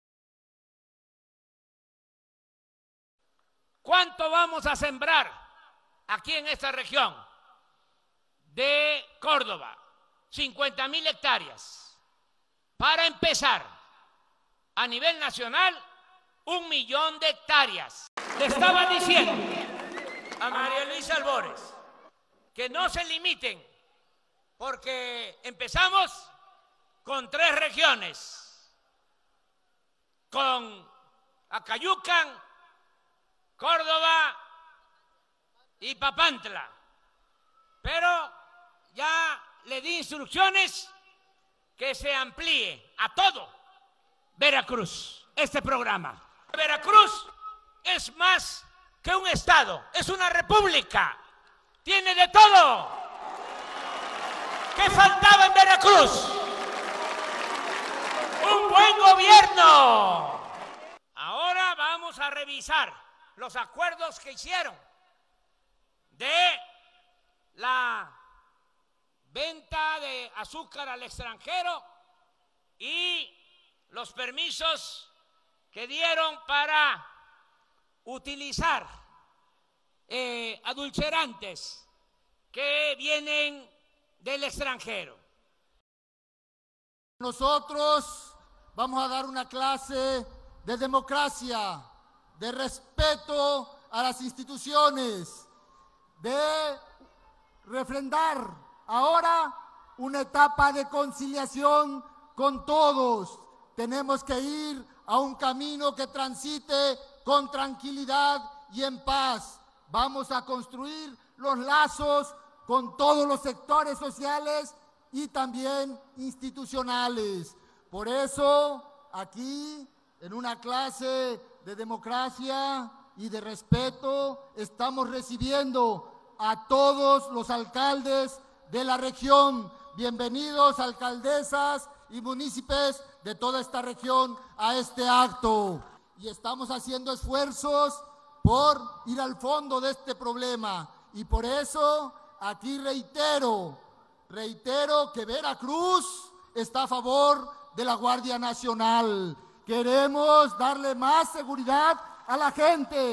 Instruye López Obrador ampliar el programa “Sembrando Vida” a todo Veracruz
*Para Córdoba, 20 mil empleos permanentes
*Se compromete a revisar la situación del sector cañero
Córdoba, Ver., 03 de febrero de 2019.- El presidente Andrés Manuel López Obrador, acompañado por el gobernador Cuitláhuac García Jiménez, instruyó a la secretaria de Bienestar, María Luisa Albores González, ampliar el programa “Sembrando Vida” a todo Veracruz.
Actualmente, la estrategia abarca las regiones de Acayucan, Papantla y Córdoba, esta última, con 50 mil hectáreas de cultivo para generar 20 mil empleos permanentes, de manera que se apoye a las familias de los ejidatarios y pequeños productores.
Asimismo, se comprometió a revisar los acuerdos de la venta de azúcar al extranjero y los permisos para el uso de edulcorantes, a fin de que los cañeros tengan cada vez más beneficios.
Dijo que el campo es la fábrica más importante de México, pero se tenía en abandono. “Ahora vamos a regresar al campo para que el veracruzano, el mexicano, pueda trabajar donde nació y ser feliz donde están sus familiares, donde están sus costumbres, donde está su cultura”.
A su vez, el Gobernador subrayó que la actual administración dará una clase de democracia, construyendo lazos con todos los sectores sociales e institucionales, en una etapa de reconciliación, y se comprometió a concluir en breve el Censo del Bienestar para no fallarle a Veracruz y combatir así el rezago de la población más necesitada.
Refrendó el respaldo de Veracruz a la Guardia Nacional para mejorar las condiciones de seguridad en la entidad, y destacó el trabajo que han realizado los diputados federales, en apoyo a la propuesta del Presidente de la República.
“Sembrando Vida” plantea un pago directo a los campesinos, de 5 mil pesos mensuales, además de asistencia técnica a las comunidades para el cultivo de árboles frutales y maderables, de acuerdo con la vocación del suelo y el clima de la región, destacando en Córdoba la plantación del café.
En presencia del subsecretario de Inclusión Productiva y Desarrollo Rural Territorial de la Secretaría de Bienestar, Javier May Rodríguez, y la alcaldesa de Córdoba, Leticia López Landero, el mandatario federal hizo entrega simbólica de tarjetas a beneficiarios de los programas “Sembrando Vida” y “Jóvenes Construyendo el Futuro”.